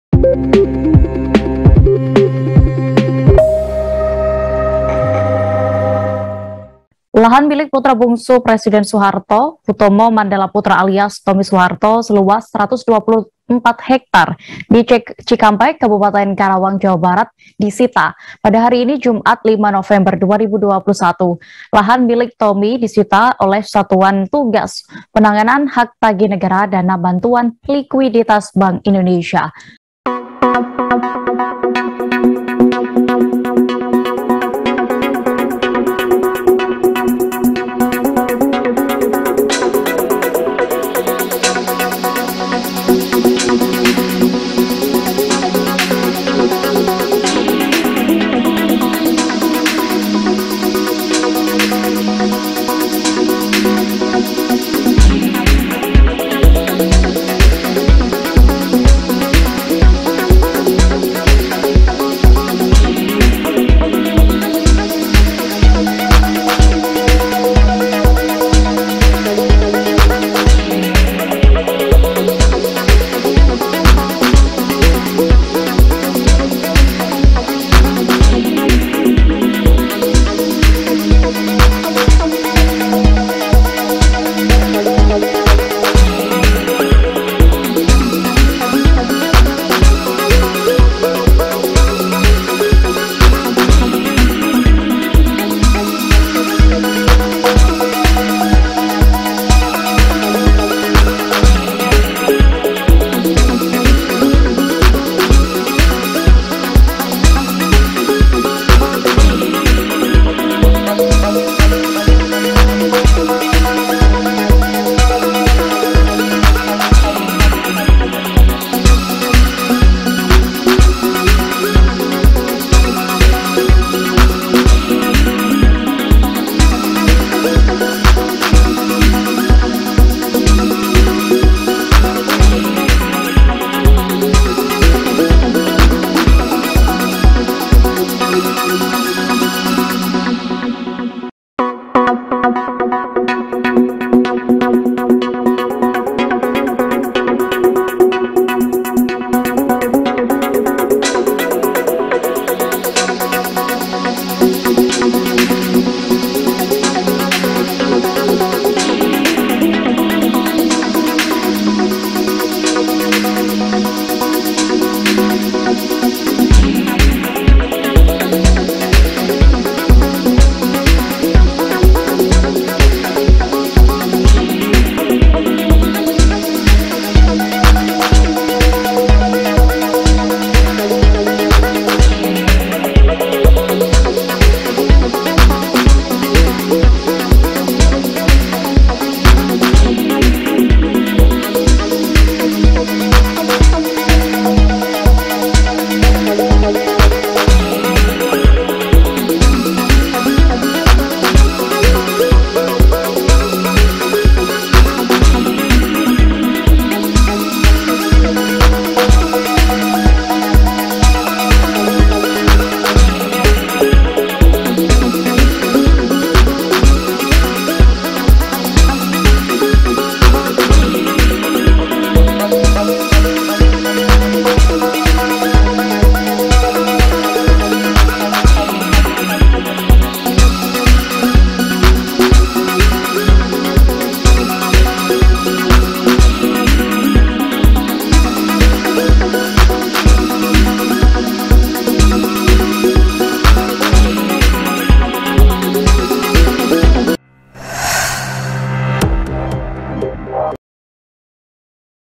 Disita karena Rugikan Negara, Satgas BLBI Eksekusi Lahan Milik Tommy Soeharto di Cikampek
Lahan milik putra bungsu Presiden Soeharto, Hutomo Mandala Putra alias Tommy Soeharto disita pada Jumat (5/11/2021).

Lahan Seluas 124 hektar itu berada di Cikampek, Kabupaten Karawang, Jawa Barat,

Lahan milik Tommy disita oleh Satuan Tugas (Satgas) Penanganan Hak Tagih Negara Dana Bantuan Likuiditas Bank Indonesia (BLBI).

Ketua Satgas Penanganan Hak Tagih Negara Dana BLBI Rionald Silaban mengatakan, aset tersebut terbagi dalam empat sertifikat.

"Lebih rincinya, nanti Pak Mahpud akan melakukan statmen (Menteri Koordinator Bidang Politik, Hukum, dan Kemananan RI, Mahfud MD)," katanya